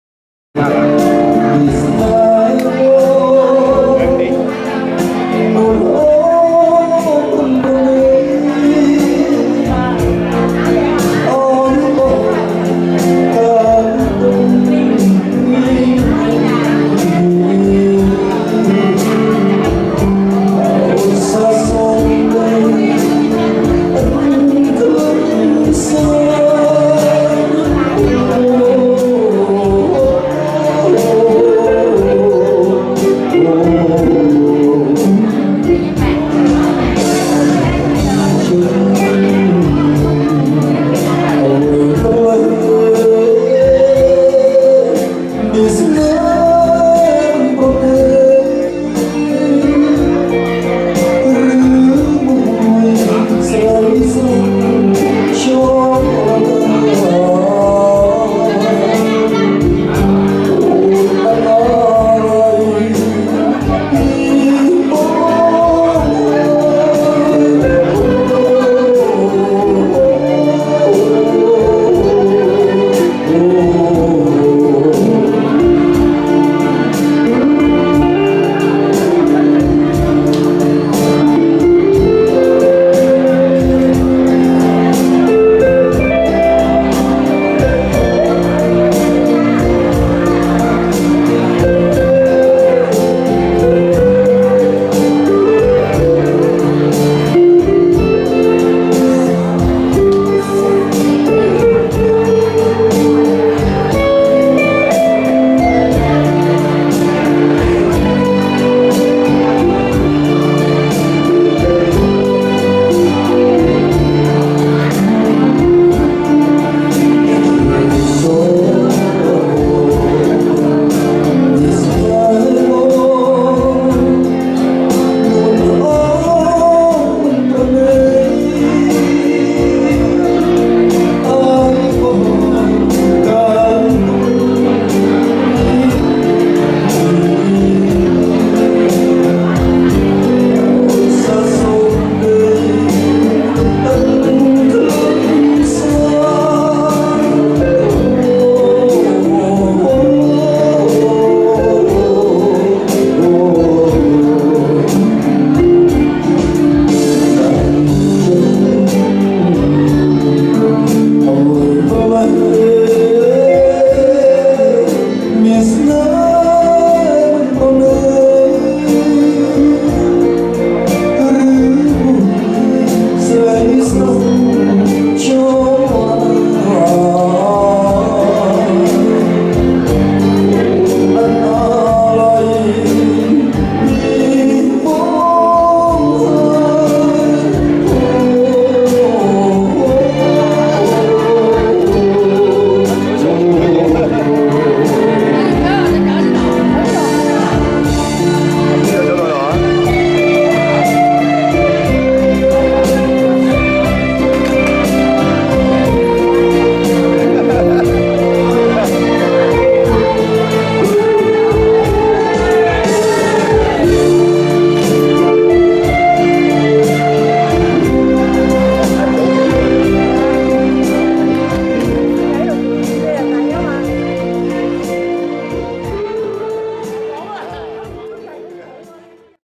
FATHER'S DAY SLOW WALTZ 2
Dancing the slow waltz at Legend Seafood restaurant.